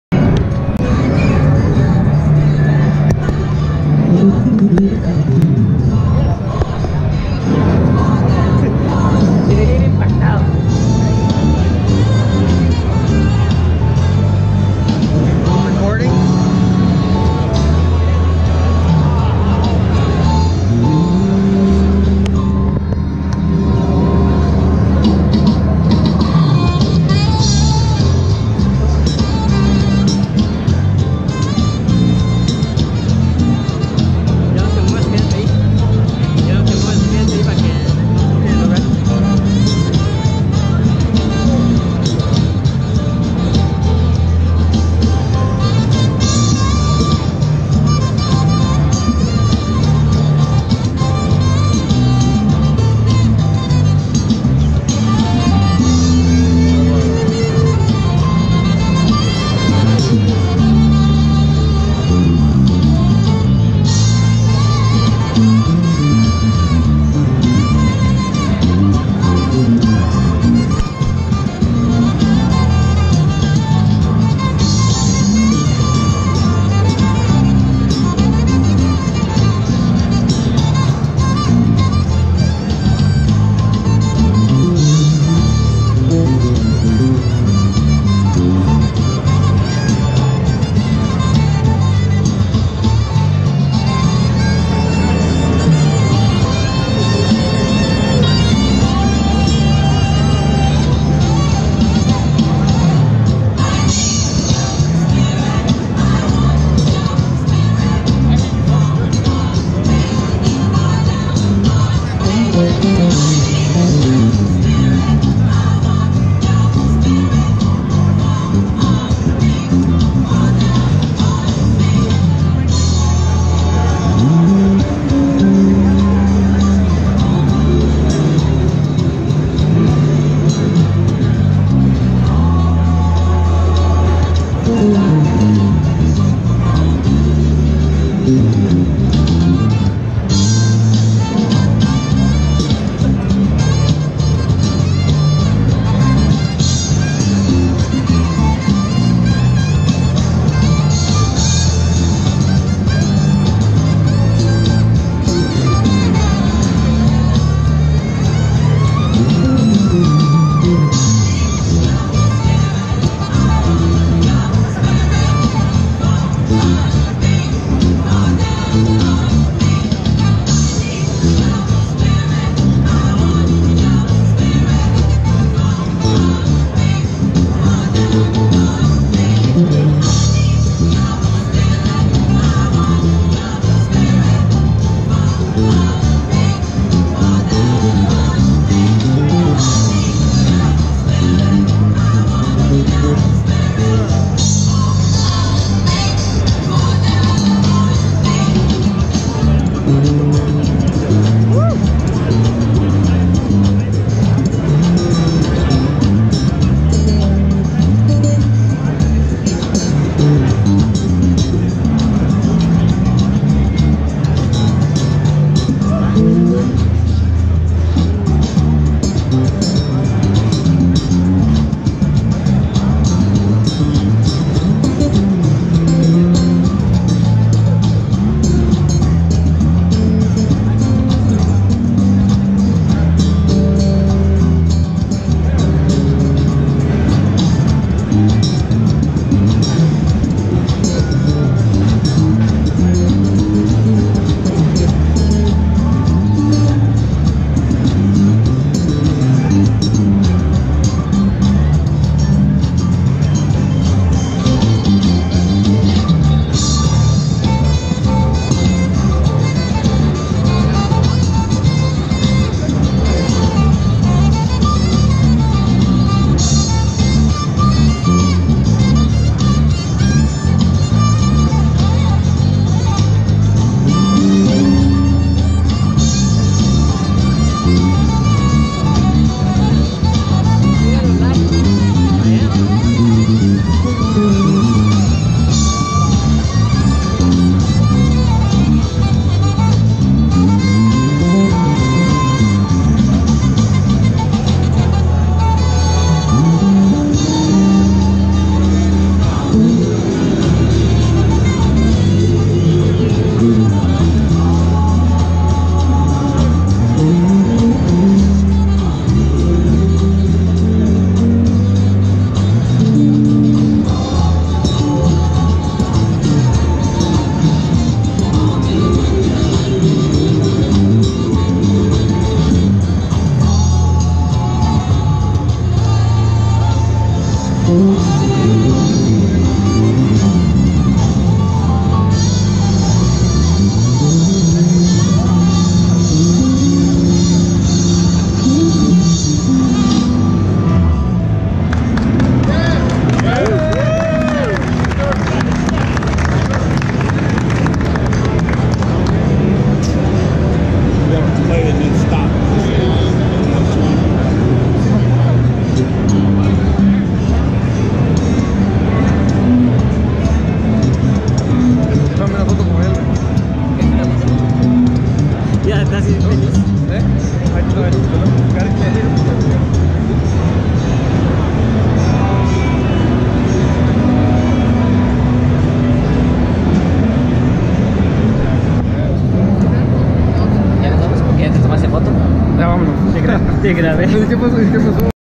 Andrew Gouche Namm 2019
Andrew Gouche playing at  Mtd both at namm show 2019